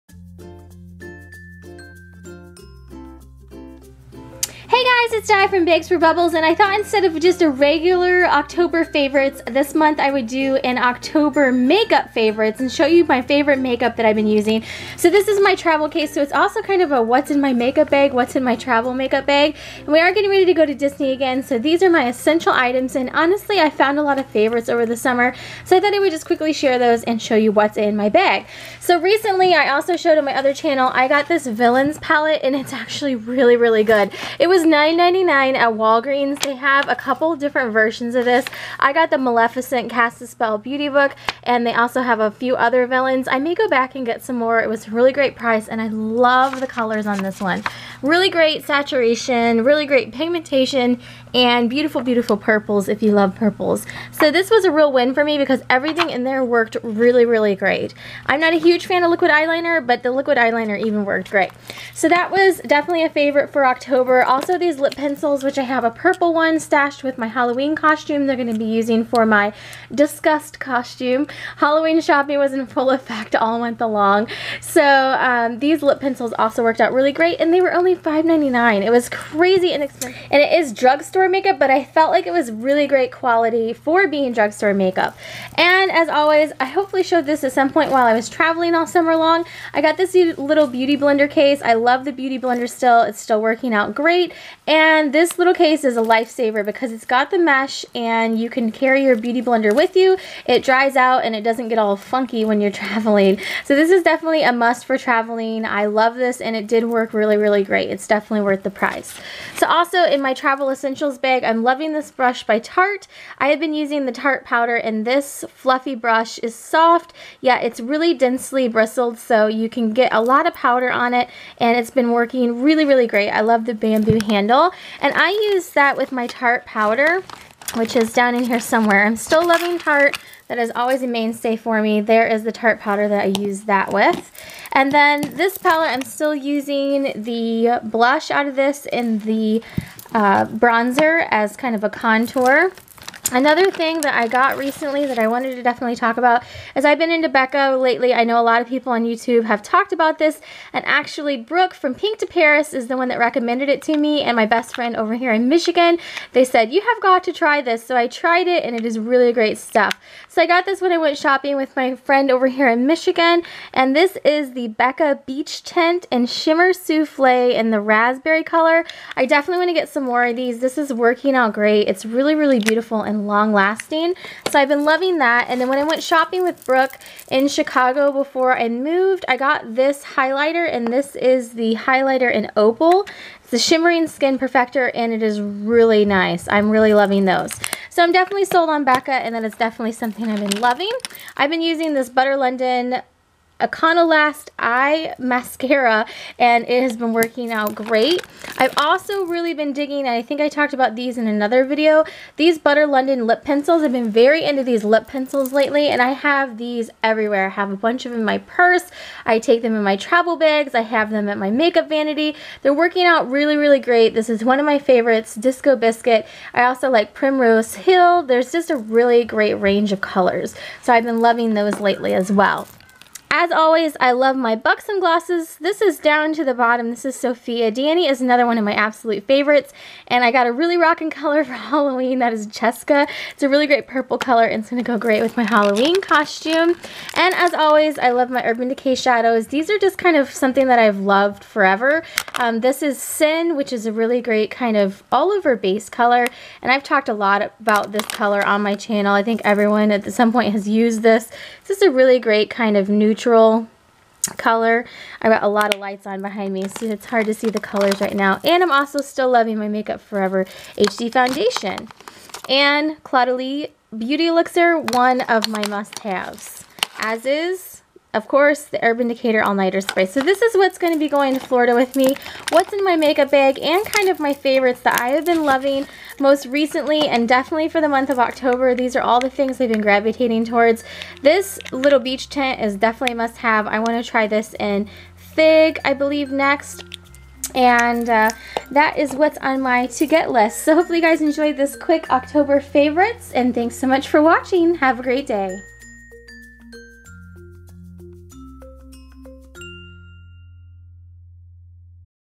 Makeup Favorites & What's in my Travel Makeup Bag!
For my October 2015 Favorites... I thought I would change it up and share my MAKEUP favorites! And also what is inside my travel makeup bag! We are headed out again to WDW and these are my essentials that I've been loving lately and will also take on my trip with me!

Snail Mail:
Diana - Bags4Bubbles